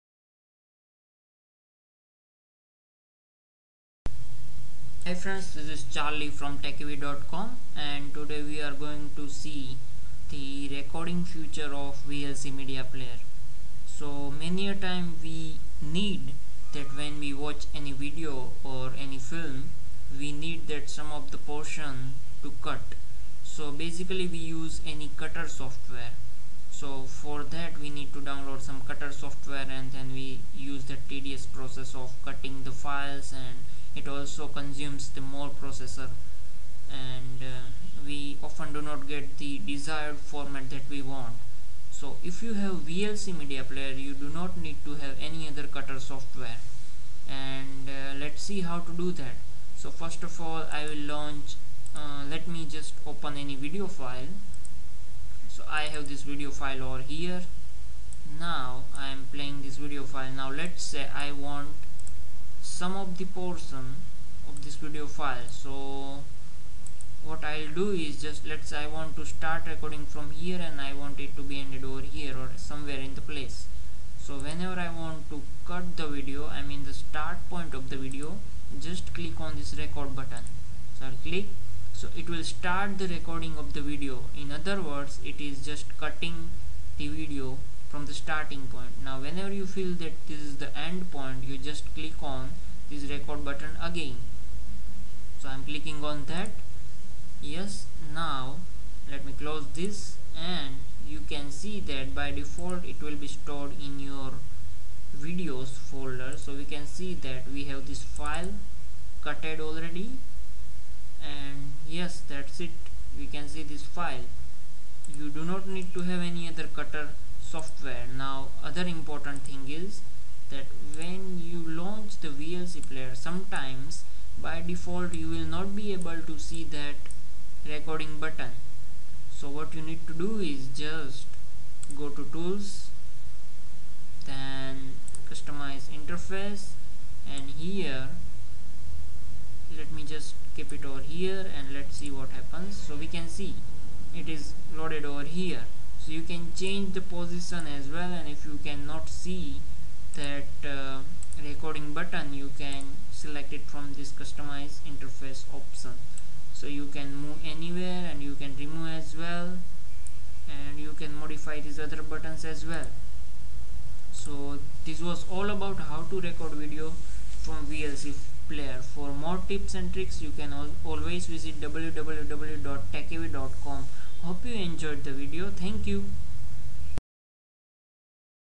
How to crop video or record audio with VLC media player - video by TechyV
You can easily cut a video using VLC media player without the need of any other video cutters. To do this, just launch VLC and play the video you want to cut.

Then click the red button which is called the record button and after that select the place you want to end your video recording and click the red button again. A file will be saved that contains only the recorded part and there you are, you have made a video cut.